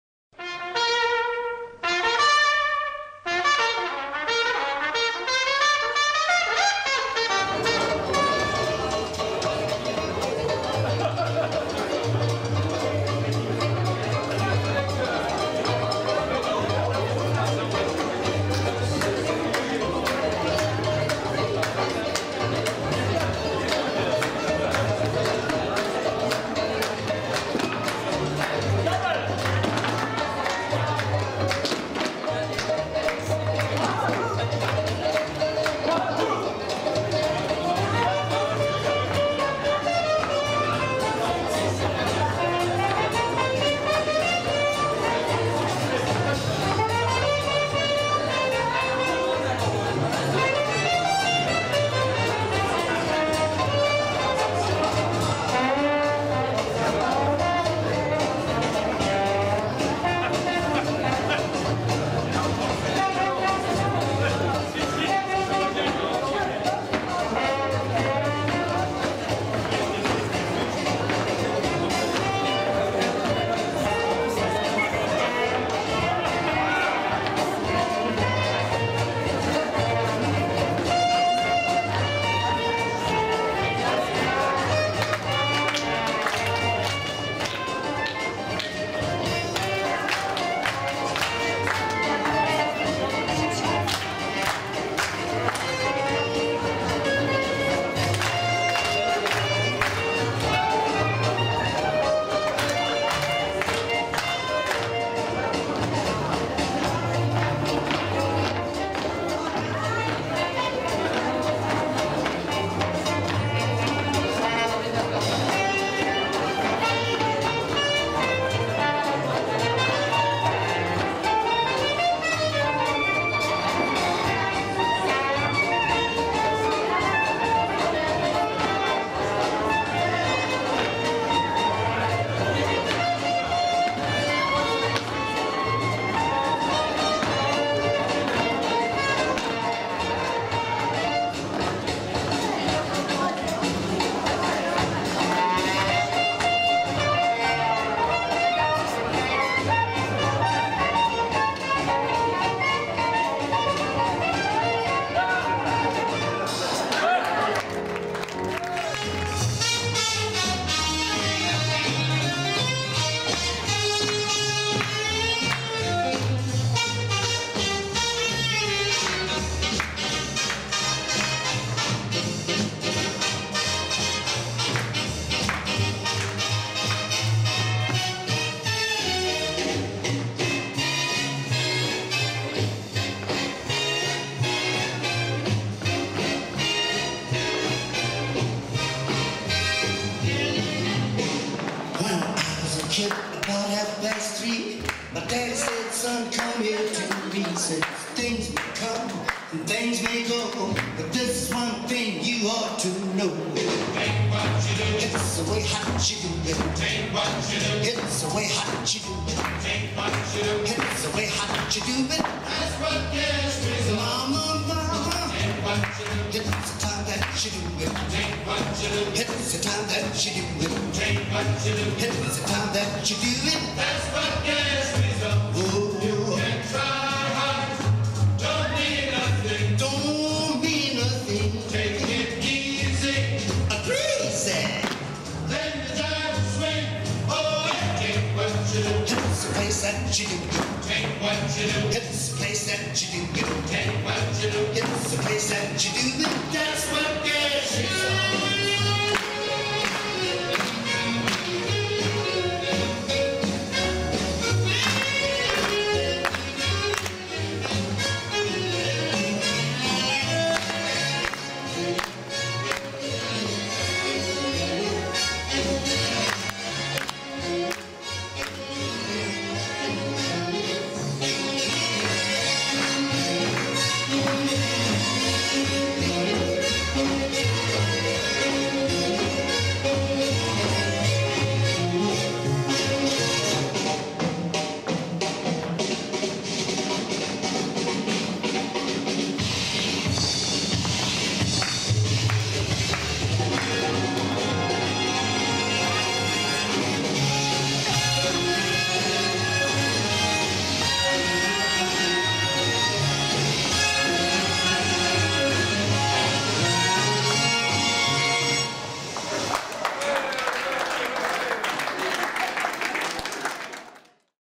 Rueda & Shim-Sham: Gala anniversaire 2018 du Lédo - 30 ans !
Rueda improvisée et Shim-Sham avec les danseurs du Lédo et de l' Ecole de danse Denis Barès. Sympa !

Le LÉDOROCK -association loi 1901 de Lons le Saunier- propose le plaisir de la découverte et de l’apprentissage des danses de salons, Rock, Lindy, Tango, Salsa, claquettes...etc.